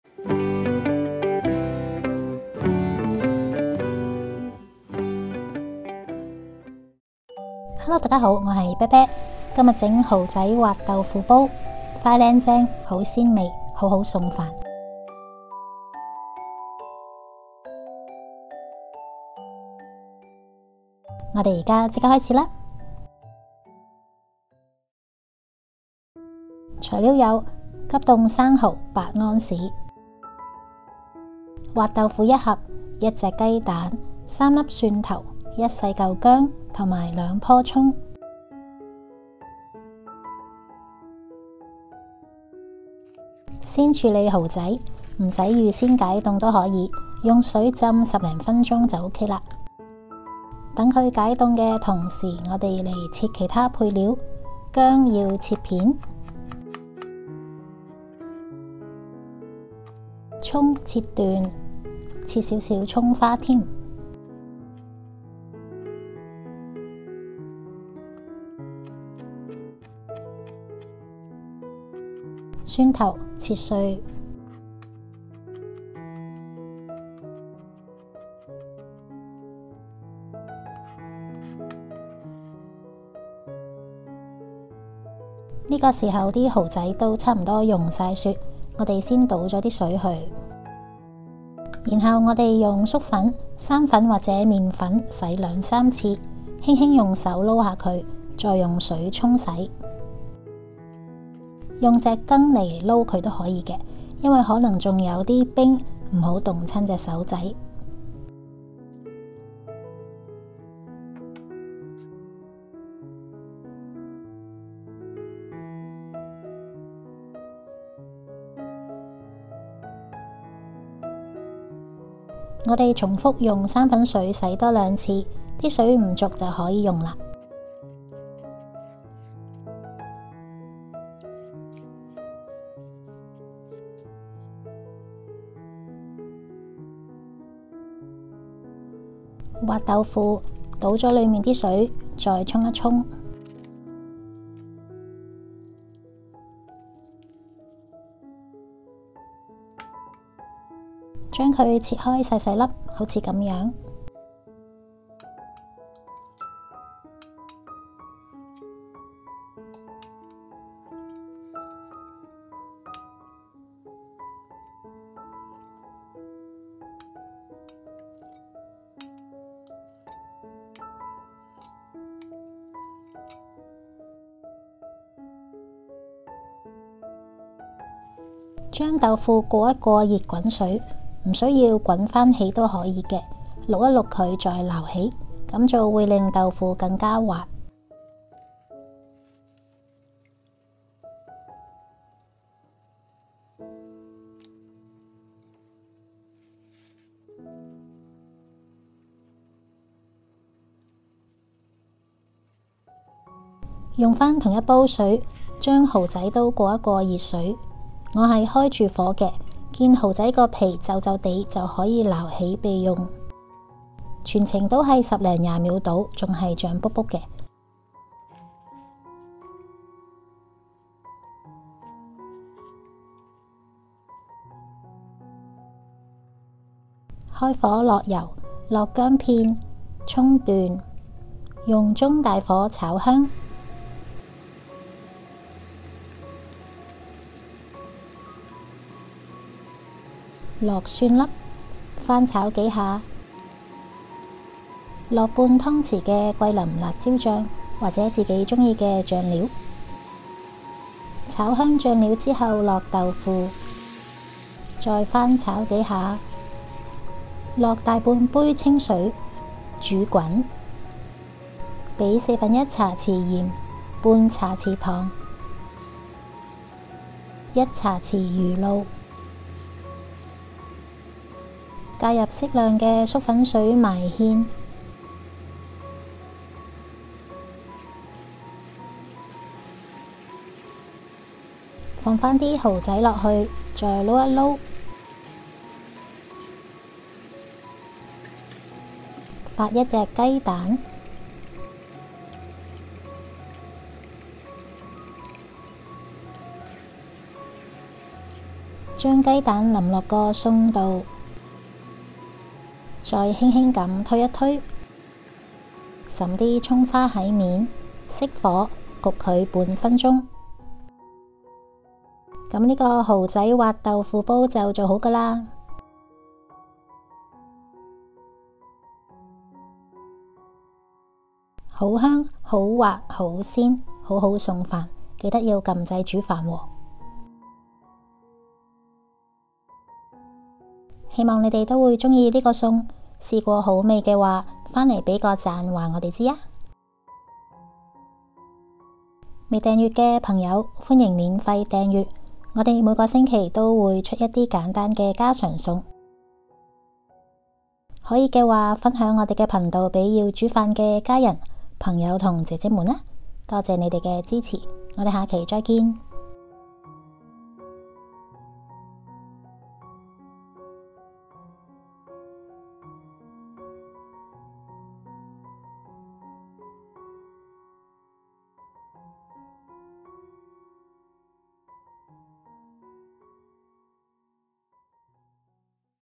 粵語 | 蠔仔滑豆腐煲 | 用急凍生蠔 | 簡單家常菜 | Tofu Stew With Oyster
Ingredients:
1 pack frozen oysters (8 oz)
1 box soft tofu (16 oz)
1 egg
3 cloves of garlic
1 small ginger
2 scallions
1/2 tbsp chili sauce
3/4 cup water
1/4 tsp salt
1/2 tsp sugar
1 tsp fish sauce
small amount of cornstarch slurry (to thicken the sauce)
材料:
1 包 急凍生蠔 (8 安士)
1 盒 滑豆腐 (16 安士)
1 個 雞蛋
3 瓣 蒜頭
1 小塊 薑
2 棵 青蔥
1/2 湯匙 桂林辣椒醬 (或自己喜歡的辣椒醬)
3/4 杯 水
1/4 茶匙 鹽
1/2 茶匙 糖
1 茶匙 魚露
少許粟粉水 (勾芡用)

我們的網上日誌